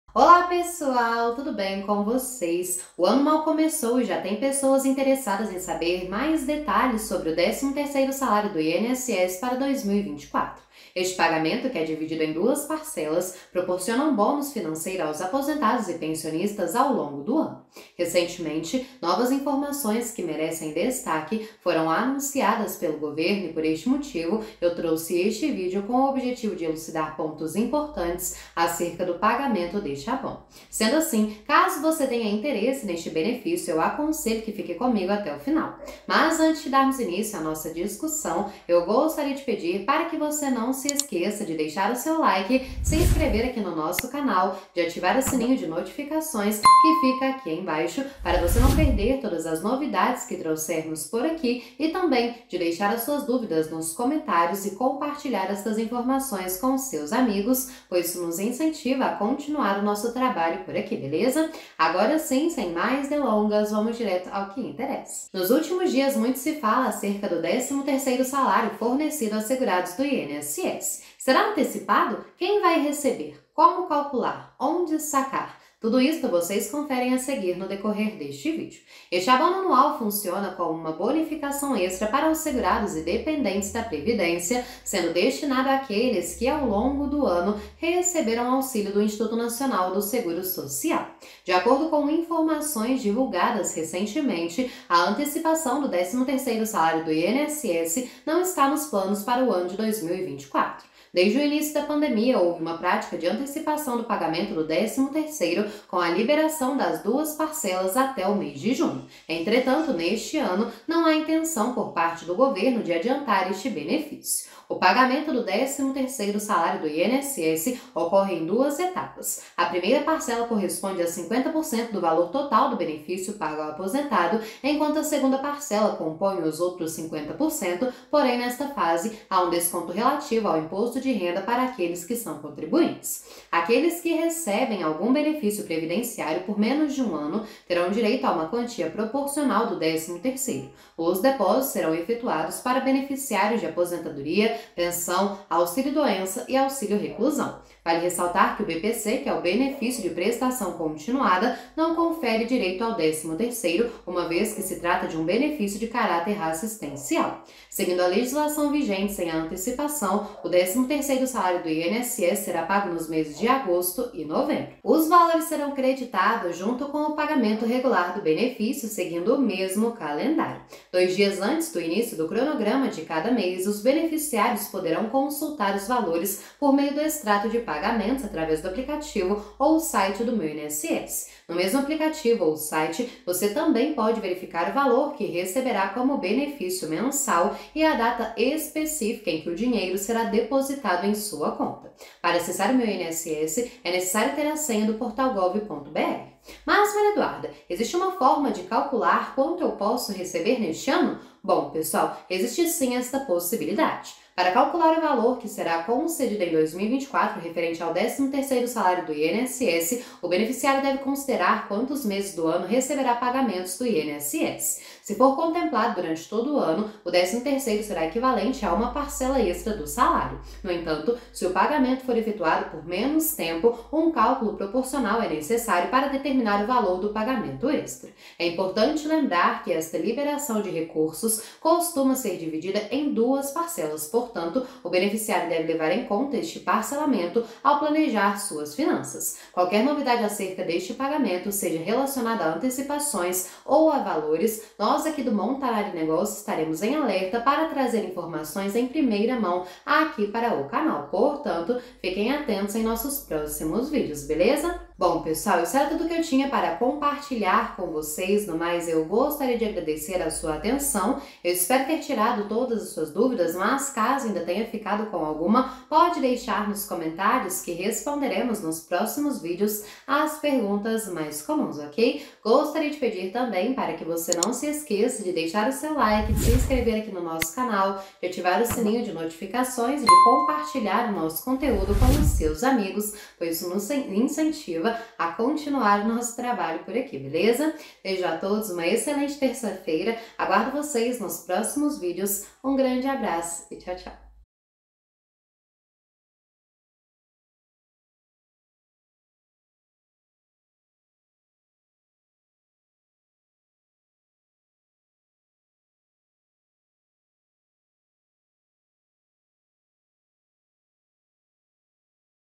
💸NOTÍCIA URGENTE! 13º DO INSS É ANTECIPADO PARA APOSENTADOS E PENSIONISTAS - VEJA O CALENDÁRIO 2024
O ano mal começou e já tem pessoas interessadas em saber mais detalhes sobre o décimo terceiro salário do INSS para 2024. Esse pagamento, que é dividido em duas parcelas, proporciona um bônus financeiro aos aposentados e pensionistas ao longo do ano.

Recentemente novas informações que merecem destaque foram anunciadas pelo governo e por esse motivo trouxe este vídeo com o objetivo de elucidar pontos importantes acerca do pagamento deste abono. Sendo assim, caso você tenha interesse neste benefício, aconselho que fique comigo até o final.